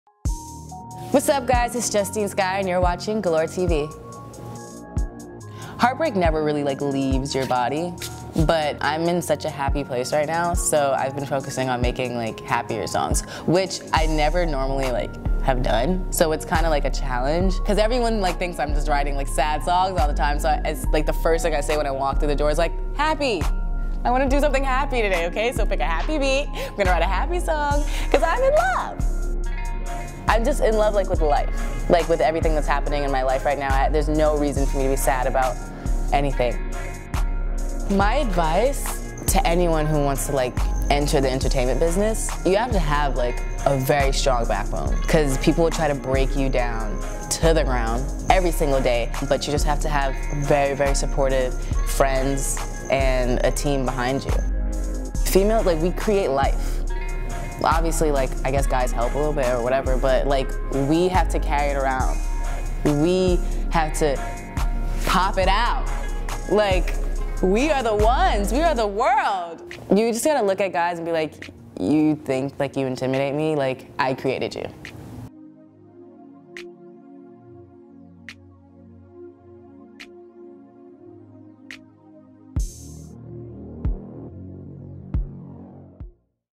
Justine Skye Is the World's Realest Unicorn | Galore TV
We talked to Purple Unicorn Justine Skye about love, new music, and the power of being a woman on set of her cover shoot.

Starring: Justine Skye
Photography by: Amber Asaly
Shot by: Maddie Cordoba
Edited by: Victoria Brandt

Get your Galore fix at…